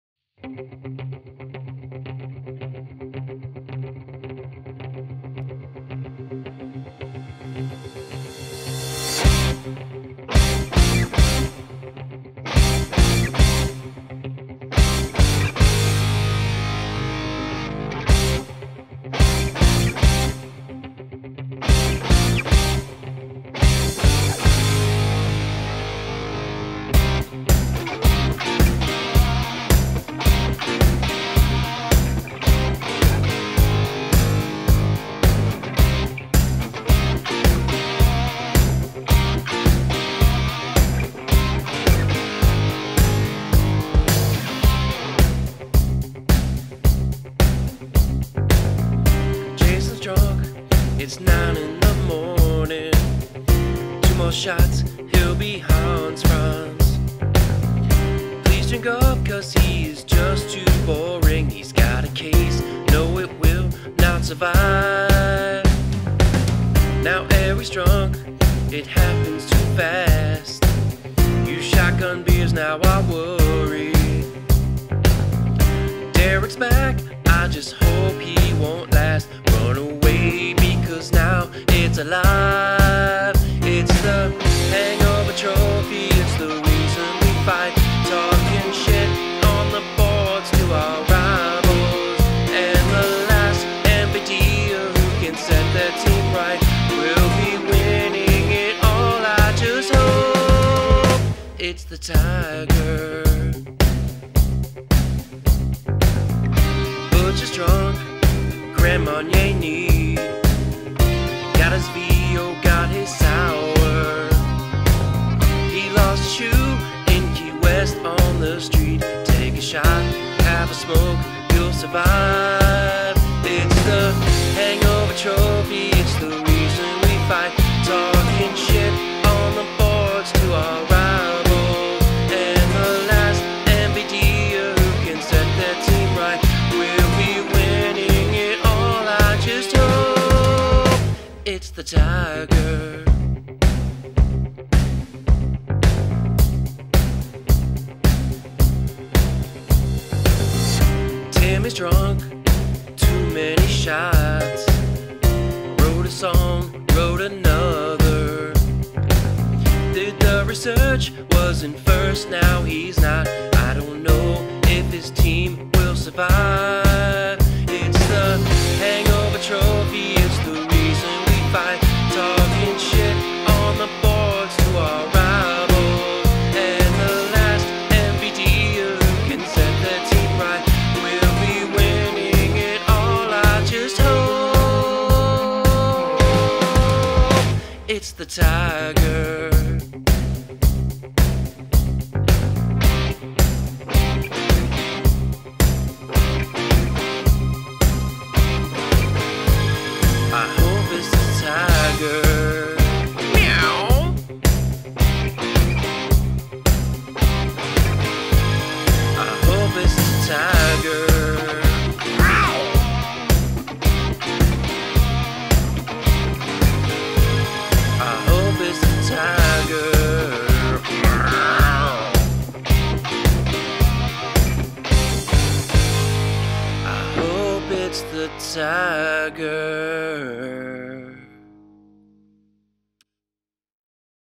MVD fantasy football playoff song
MVD (Most Valuable Drinker) fantasy football playoff teams are set and this is the official song of the 2016 season.